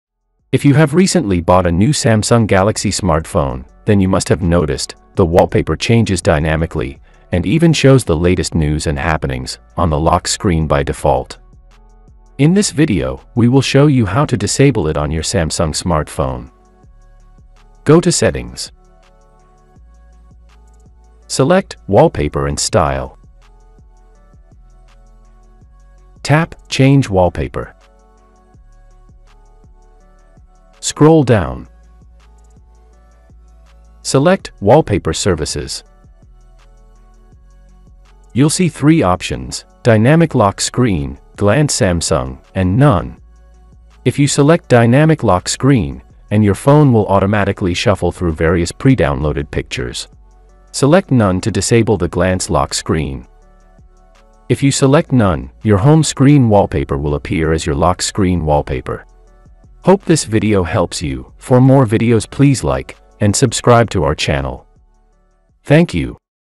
How to Turn Off Lock Screen Wallpaper Auto Change Samsung Phone
How to disable Glance lock screen on Samsung Galaxy smartphones | How to Disable Lock Screen Wallpaper services or Ads on Samsung Galaxy